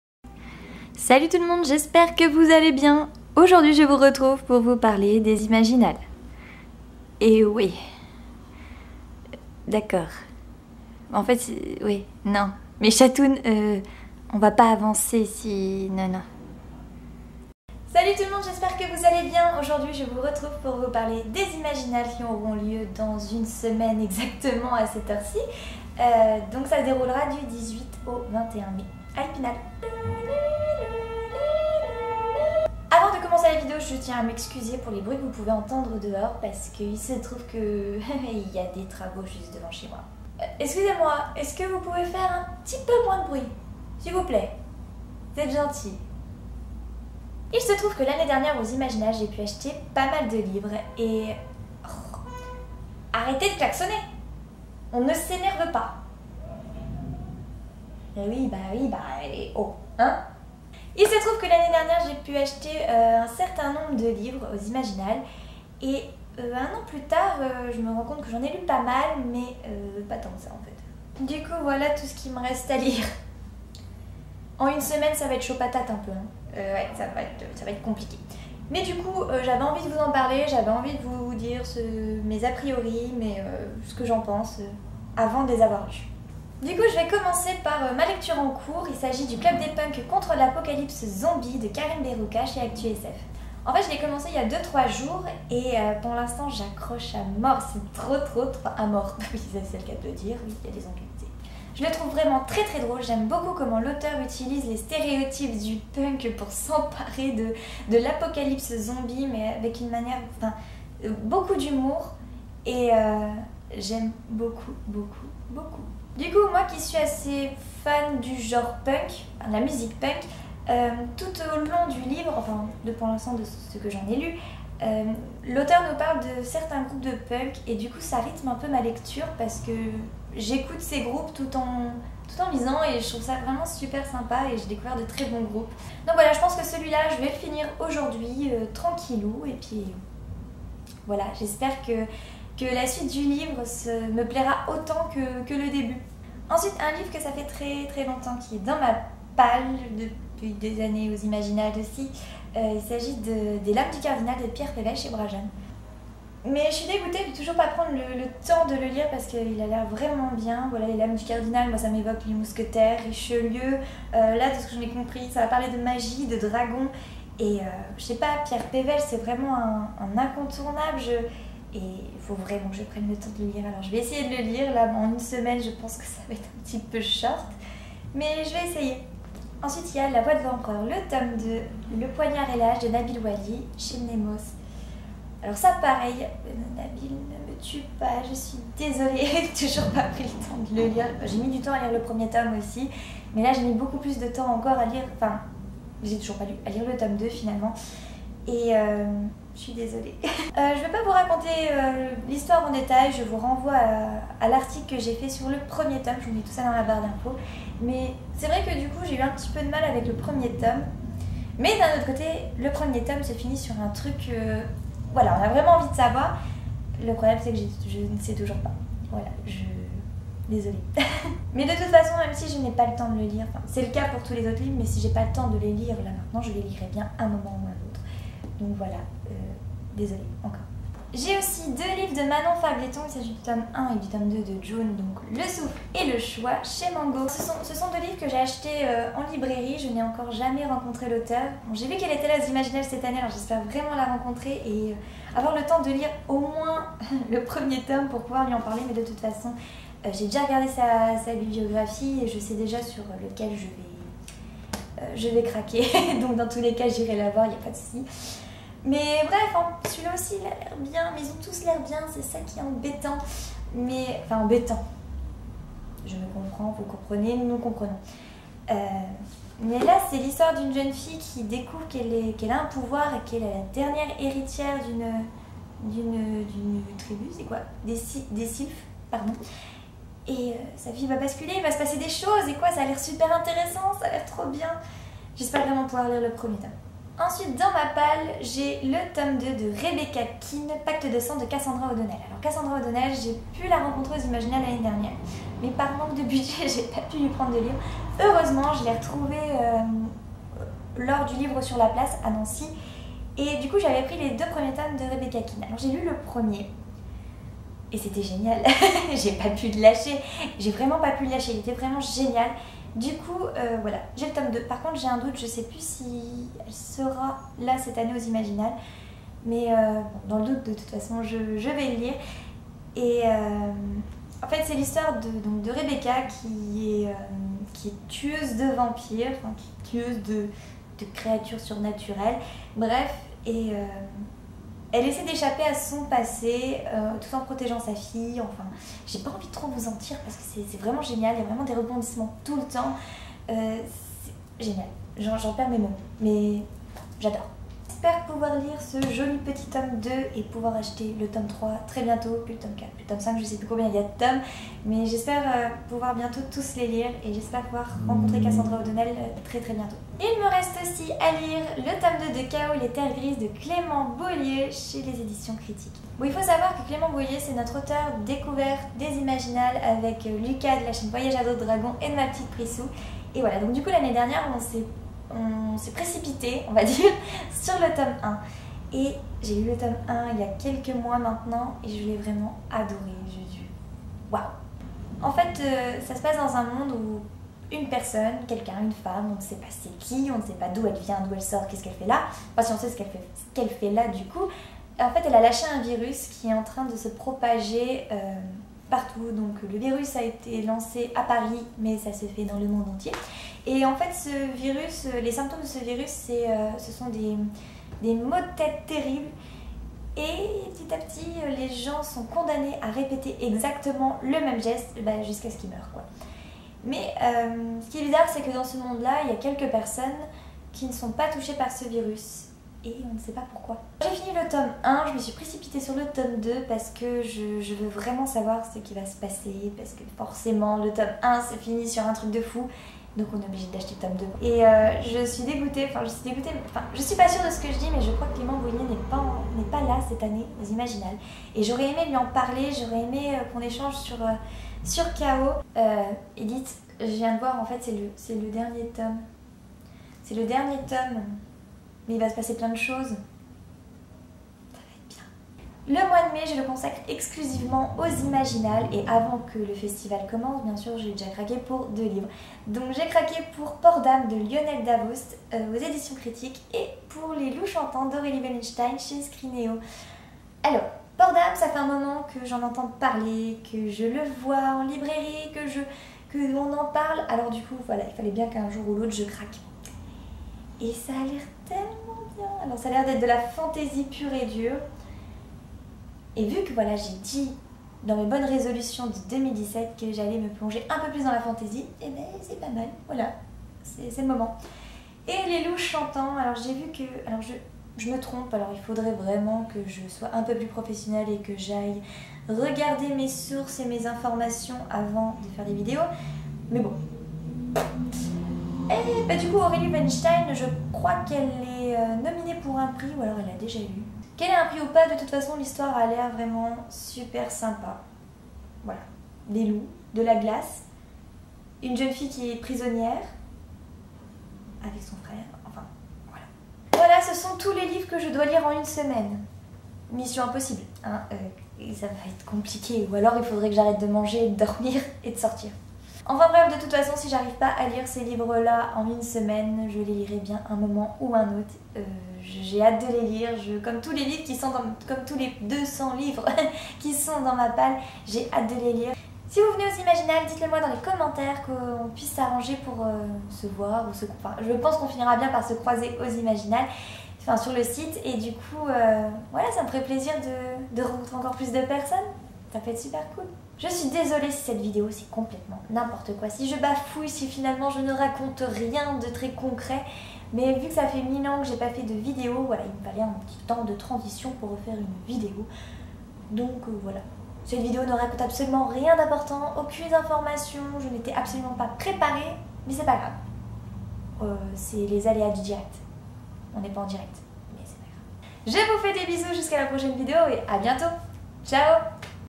P.A.L. des Imaginales 2017 (18 au 21 Mai 2017)
Il faut dérouler la barre d'infos parce qu'il s'y passe pleins de choses et il faut mettre la HD parce que c'est mieux !

Coucou tout le monde, aujourd'hui, j'avais envie de vous parler de tous ces livres que je n'ai toujours pas lu pour les Imaginales.

Livres présentés:
- Le club des punks contre l'apocalypse zombie de Karim Berrouka
- Port d'Âmes de Lionel Davoust
- Rebecca Kean (tome 2: Pacte de sang) de Cassandra O'Donnell
- Chaos (tome 2: Les terres grises) de Clément Bouhélier
- Les loups chantants de Aurélie Wellenstein
- June (Le choix et Le souffle) de Manon Fargetton
- Les lames du cardinal de Pierre Pevel
- La voix de l'empereur (tome 2: Le poignard et la hache) de Nabil Ouali

BABELIO: ShapeFleur
LIVRADDICT: Shape Fleur